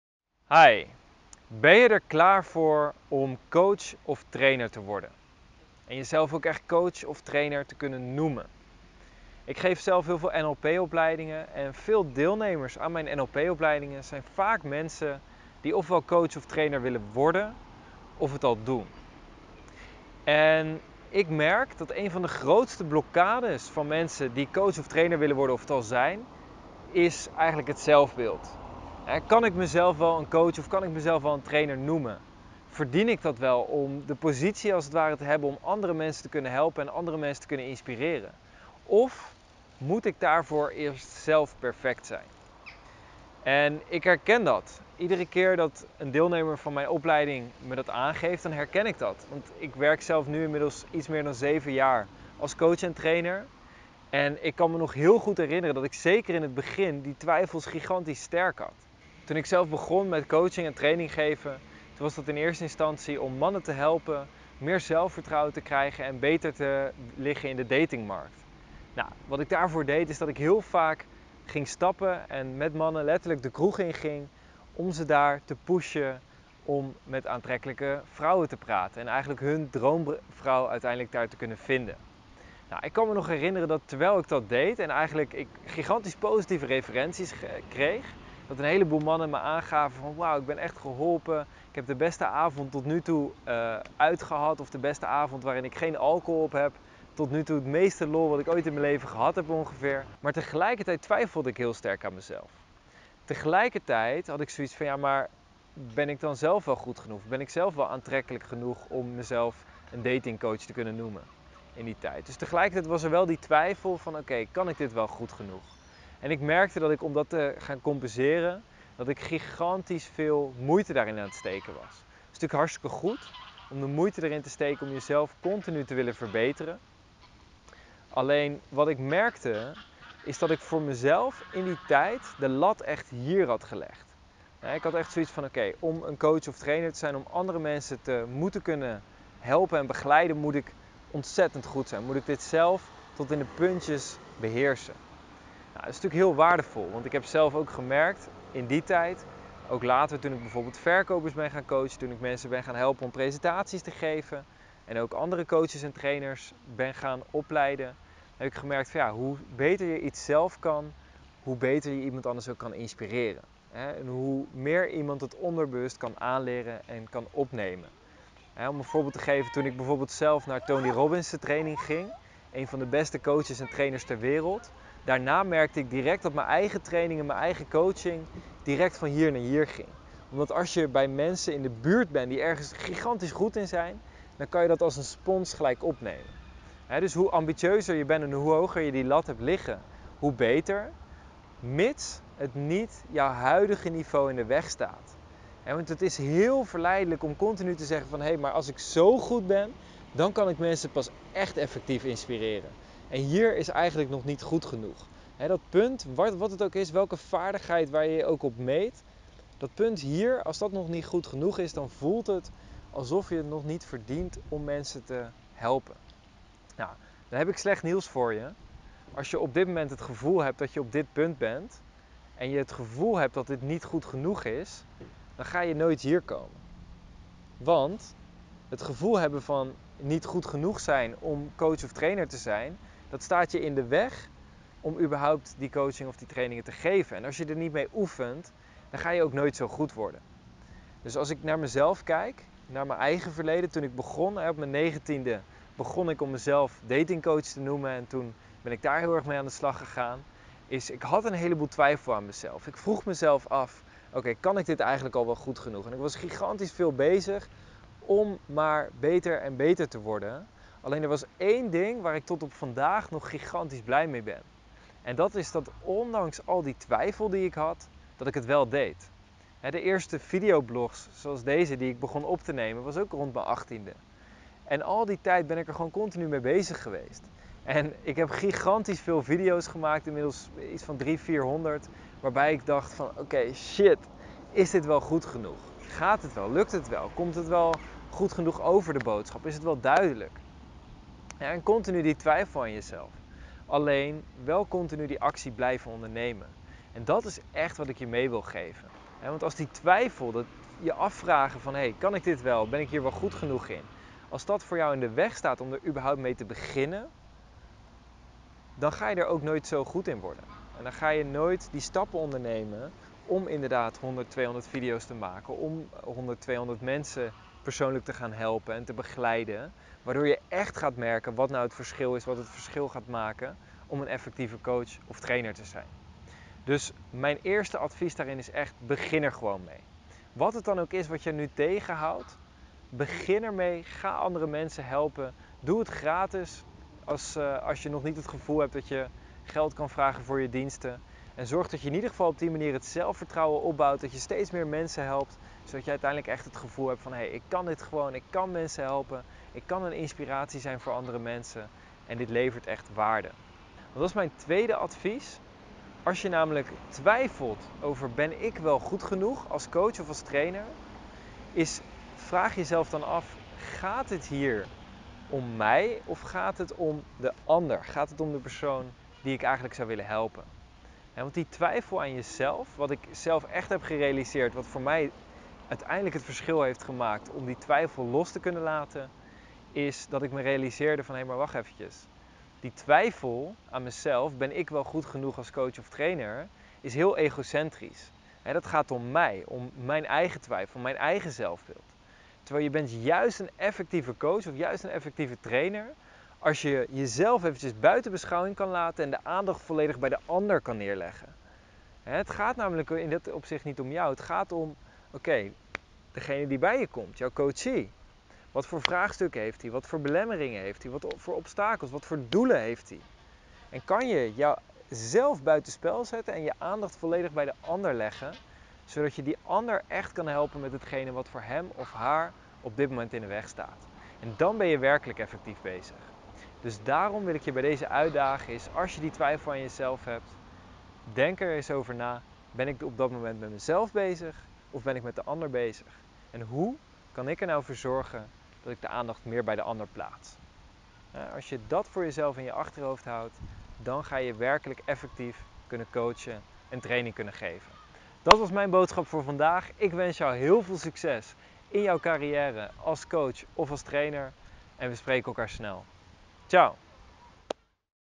Ben Jij Klaar Om Coach of Trainer te Worden?
Meld je aan voor de Transformatie Tour, een intensief en diepgaand seminar om het maximale uit jezelf te halen:

Als NLP Trainer leid ik regelmatig mensen op die de ambitie hebben om coach of trainer te worden. Een grote vraag die deze mensen vaak hebben is "Ben ik er wel klaar voor om coach of trainer te worden?"

Wanneer ben je er klaar voor?
Hoe weet je of je goed genoeg bent?

In deze video geef ik je mijn perspectief op deze vraag. Als jij de ambitie hebt om coach of trainer te worden, dan zal deze video je helpen om voor jezelf duidelijk te maken of je er klaar voor bent.